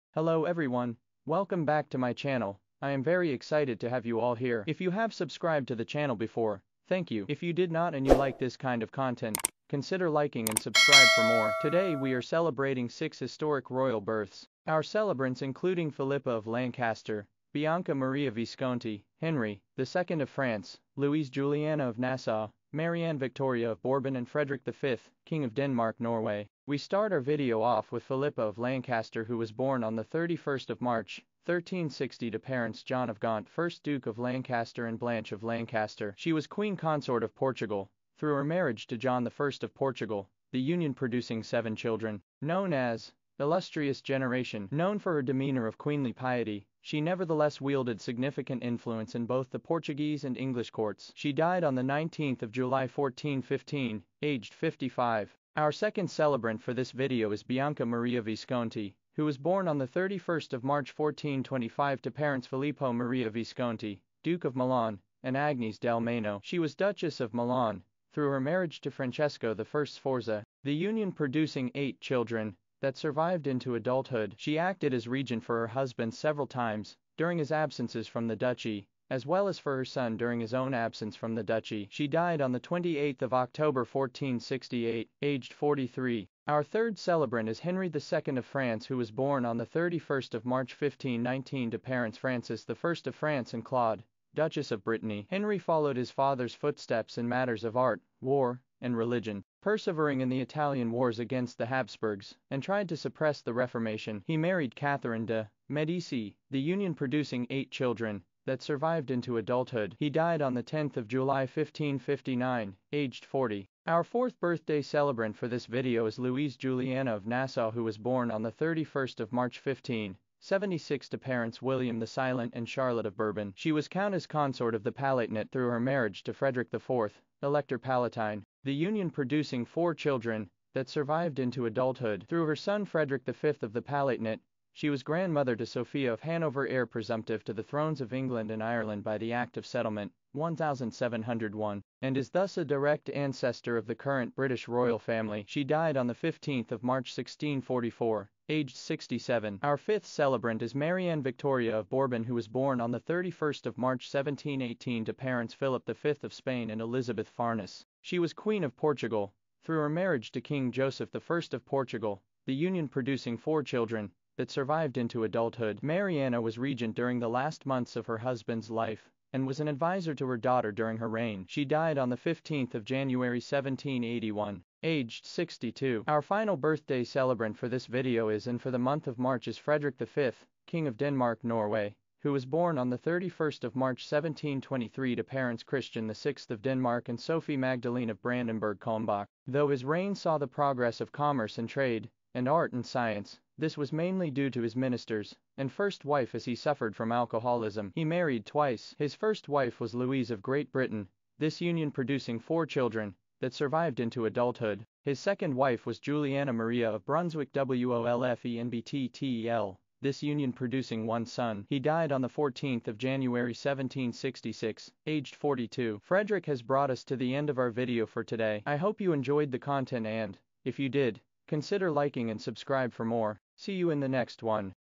Celebrating Historic Royal Birthdays for 31 March
If you would like me to do a deep dive into one of your favourite royals, leave a comment in the section down below.  If you have celebrated your birthday on the 31st of March, or know someone who has, I hope you and they had a wonderful day.